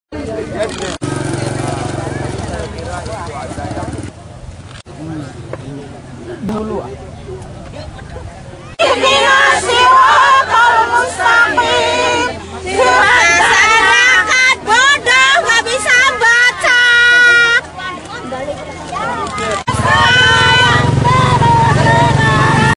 Warga masih Bertahan di Lahan Sengketa
Deli Serdang: Ratusan warga masih bertahan di lahan sengketa eks hak guna usaha PT Perkebunan Nusantara 2 Desa Ramunia, Kecamatan Pantai Labu, Deli Serdang Sumatera Utara. Sebelumnya, warga sempat bentrok dengan anggota Puskopad Kodam 1 Bukit Barisan.